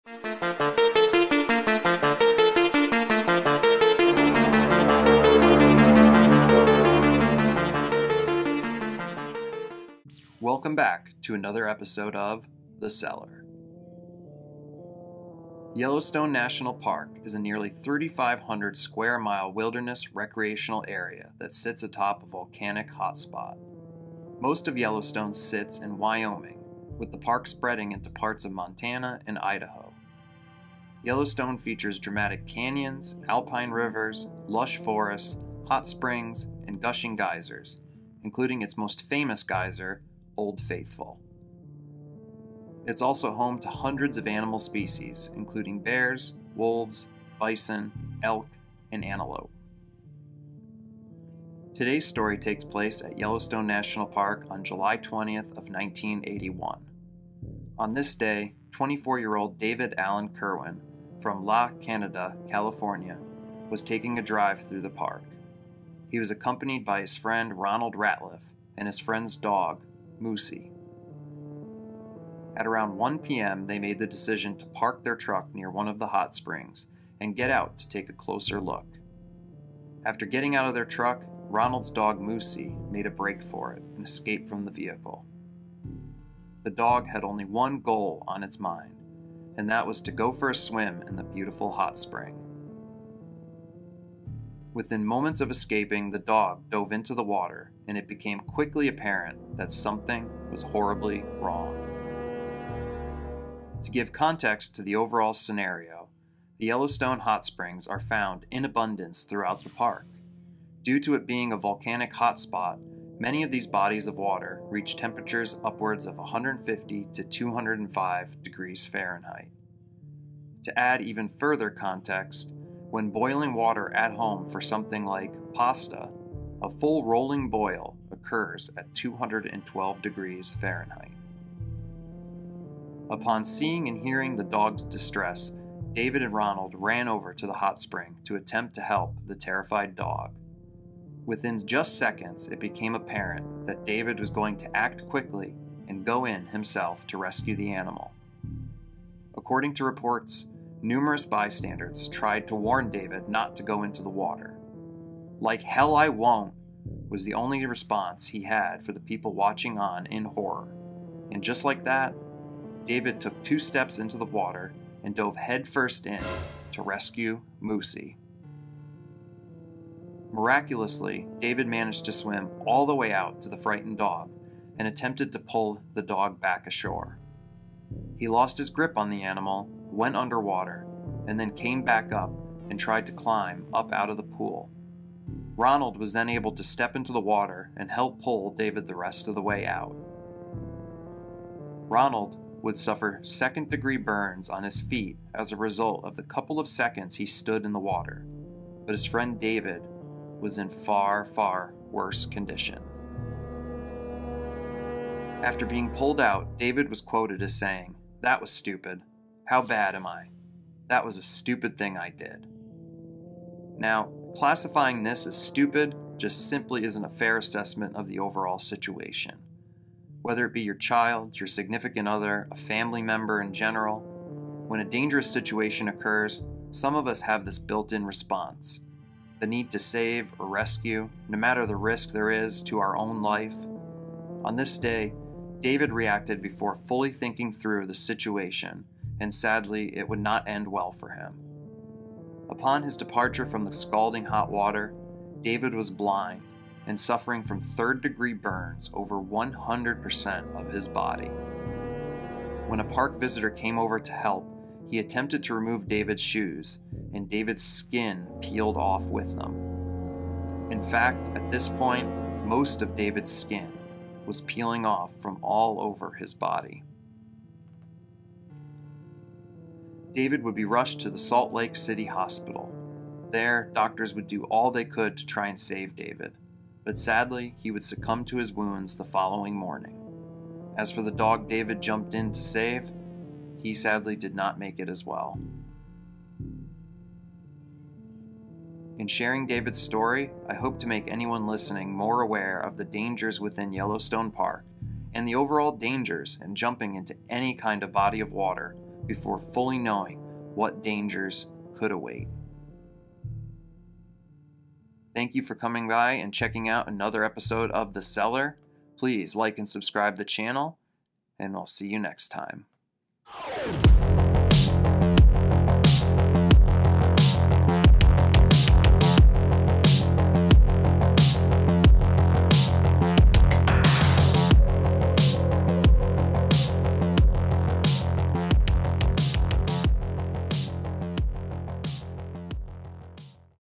Horrible Fates Tragedy At Yellowstone (David Alan Kirwan)
In todays episode I will be going over the tragic story of David Alan Kirwan at Yellowstone National Park.

Thank you for checking out the content and supporting the channel!!!